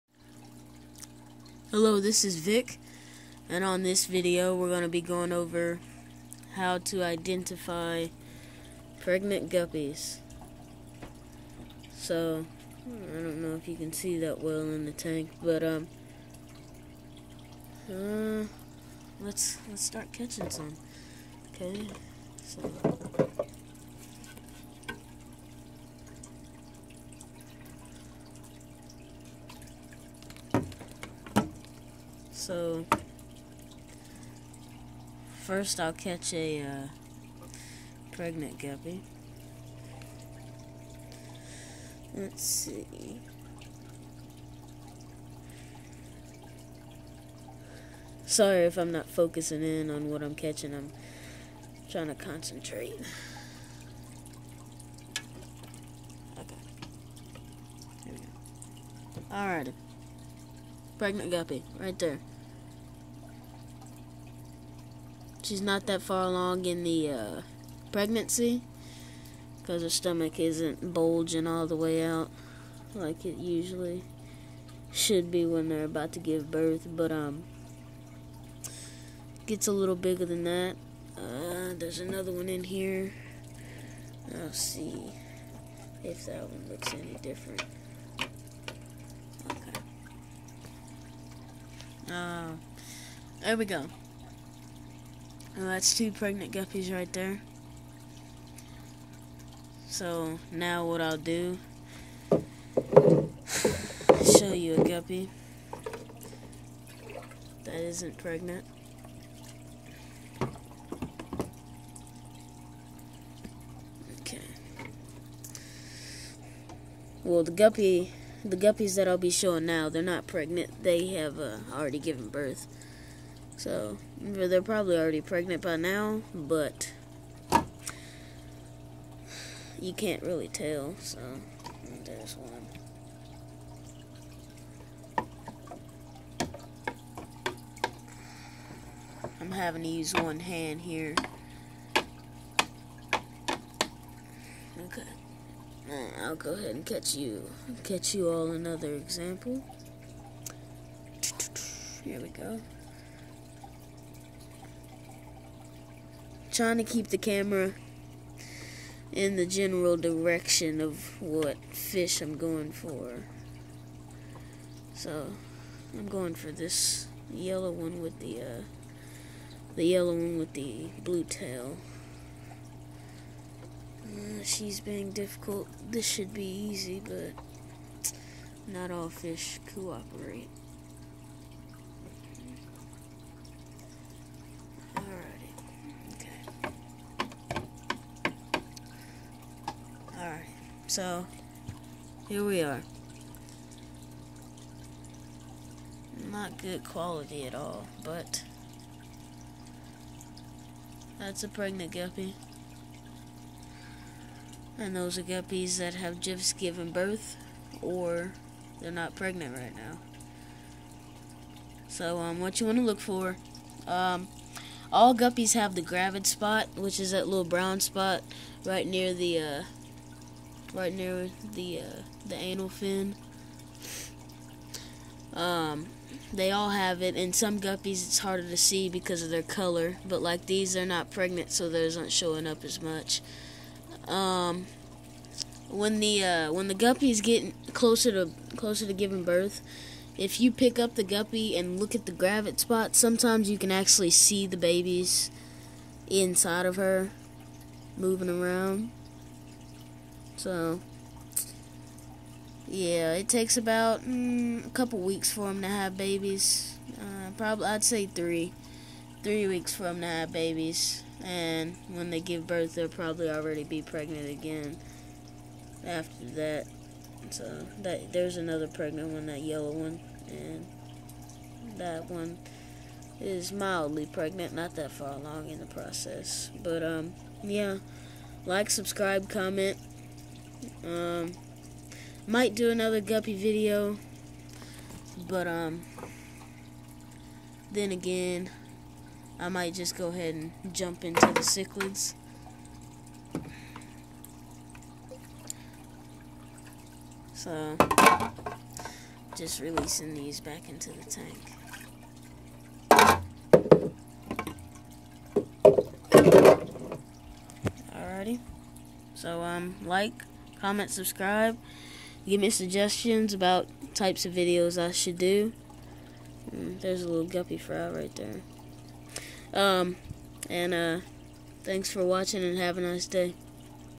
Vic's how to tell of your guppies is pregnant?
In this video I show and tell you how to identify a pregnant guppies.